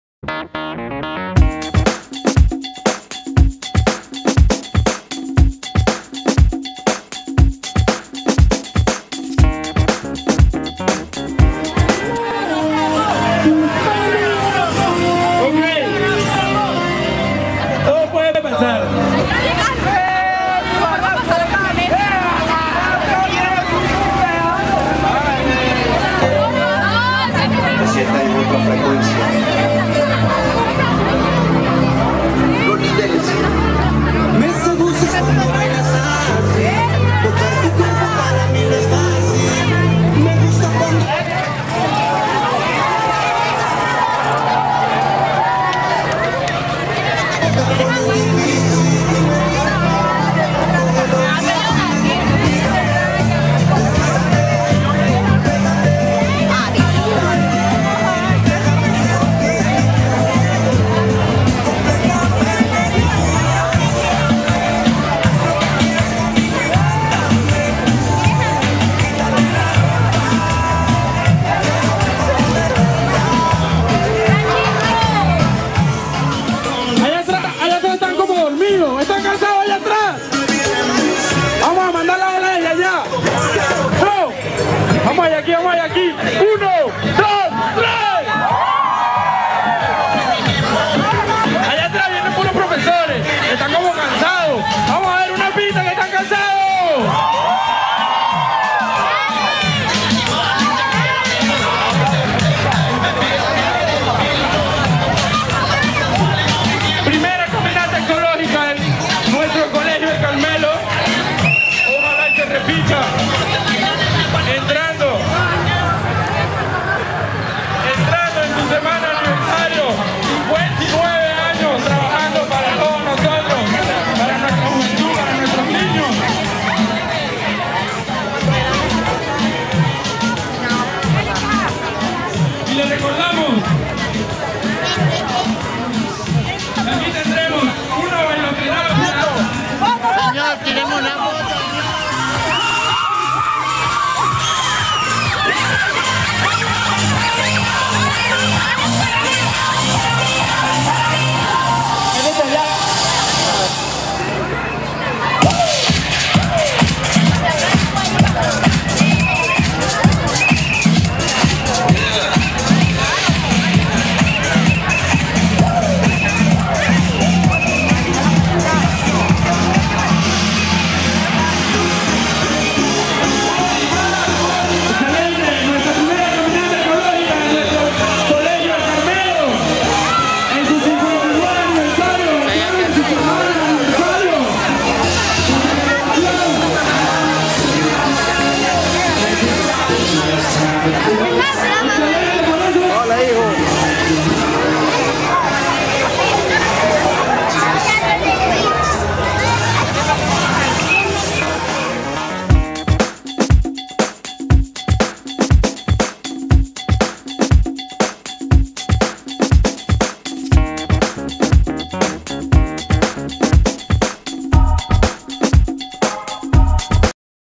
PRIMERA CAMINATA ECOLÓGICA COLEGIO EL CARMELO DE CAGUA
PRIMERA CAMINATA ECOLÓGICA COLEGIO EL CARMELO DE CAGUA, Actividad Deportiva realizada en Cagua Edo. Aragua - Venezuela el d{ia Sábado 18 de Mayo de 2013, en el marco del 59 Aniversario de la UEP Colegio "El Carmelo" de Cagua.